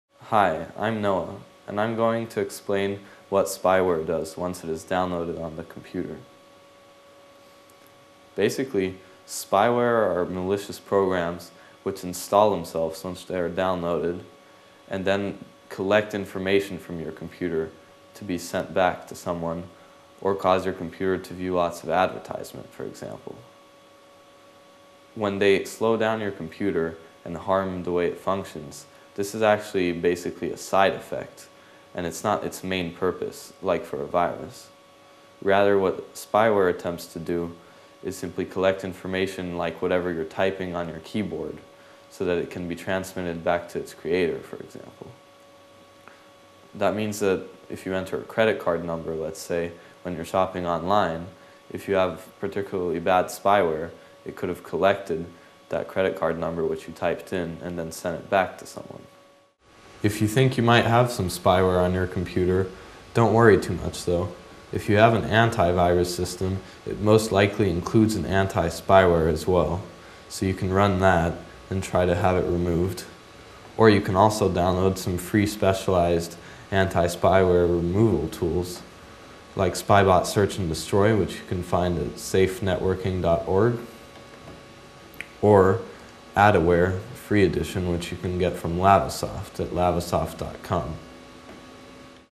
Internet & Computer Viruses : What Does Spyware Do Once It Is Downloaded on the Computer?
Once it is downloaded on a computer, spyware collects information and activity from the machine to send back to the creator of the spyware, which makes the computer run slower and be more cluttered with advertisements and pop-ups. Understand how spyware works, which can be eliminated with a good anti-virus program, with information from a freelance computer specialist in this free video on the Internet.